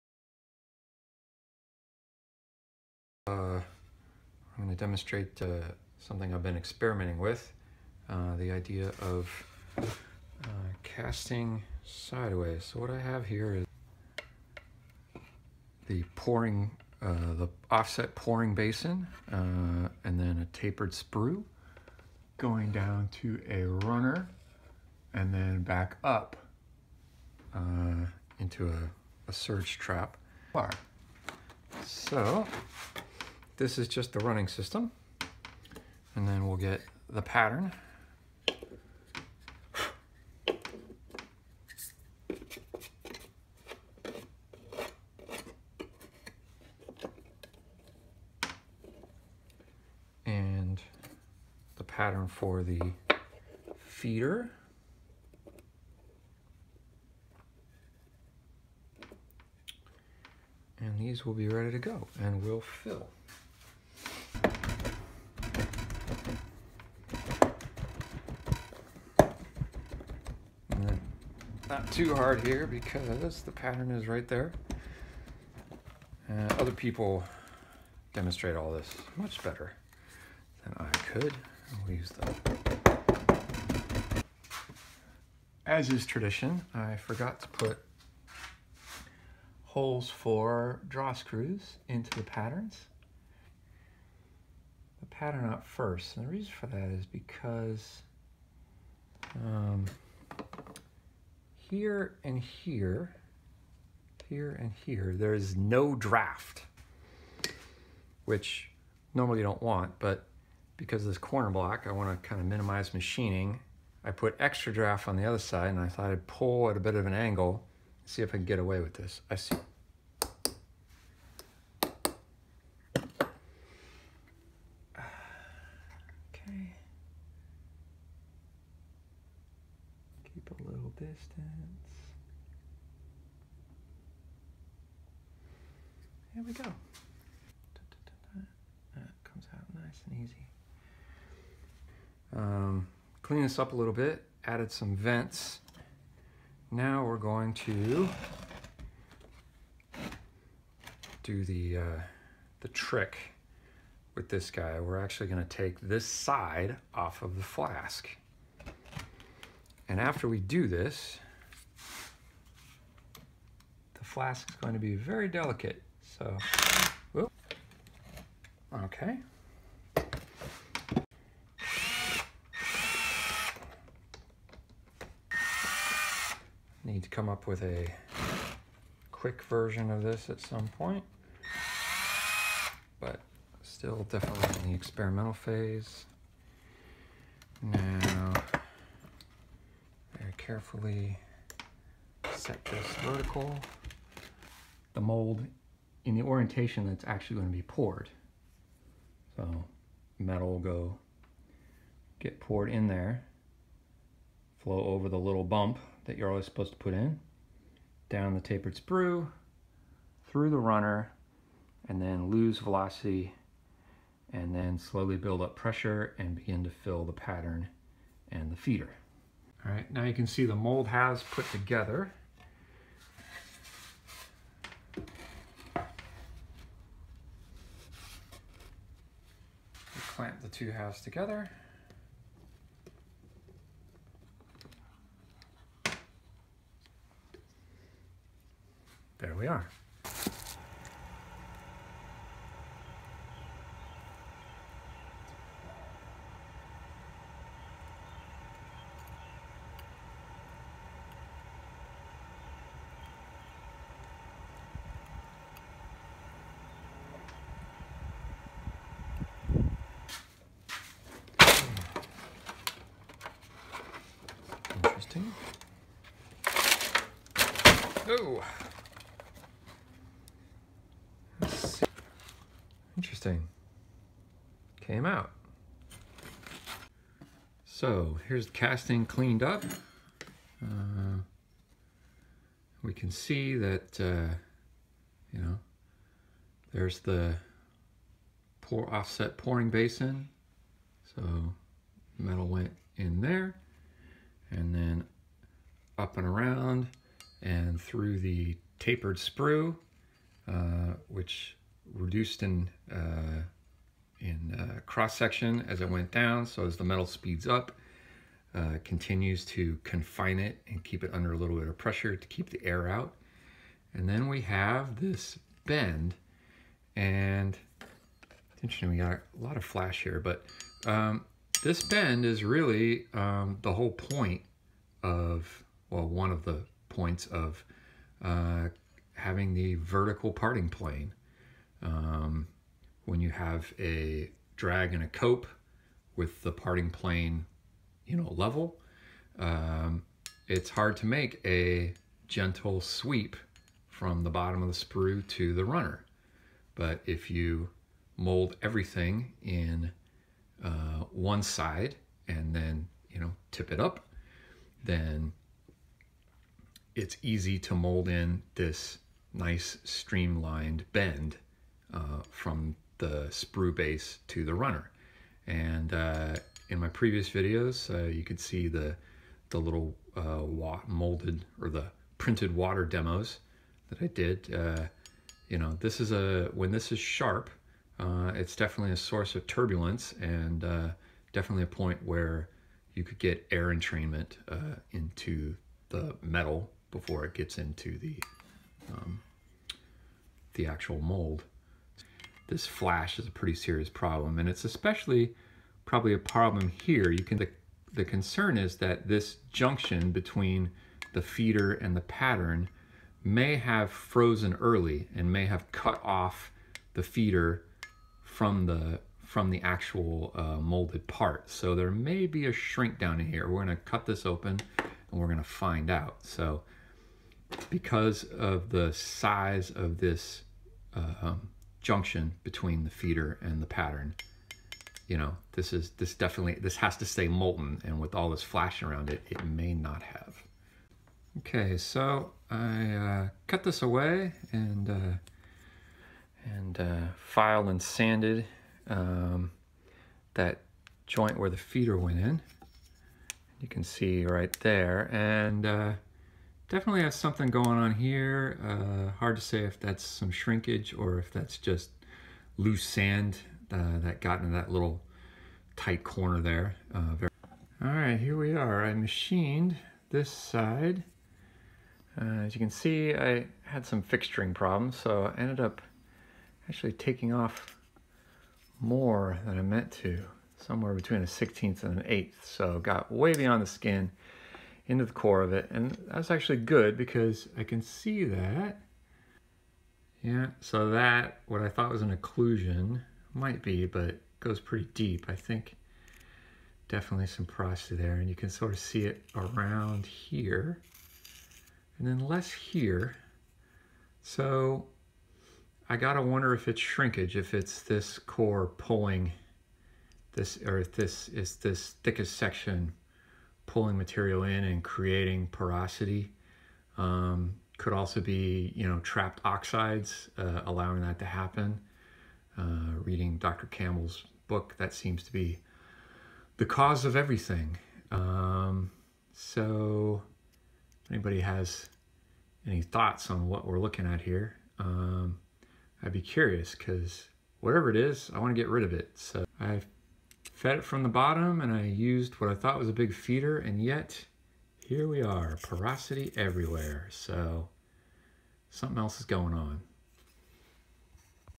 Casting with a Vertical Parting Plane
After some experimenting, I have found that it is sometimes easier to mold the parts of a running system into a single side of a modified flask with a removeable side and then tip the flask up to make the parting plane vertical. As you will see, it is still a work in progress. Advice is welcome.

Please don't watch this if you are learning the basics of sand casting. These are the guys you want to watch:

Also, thanks again to Andrew Martin. His experiments with visualizing the casting process inspired all of this: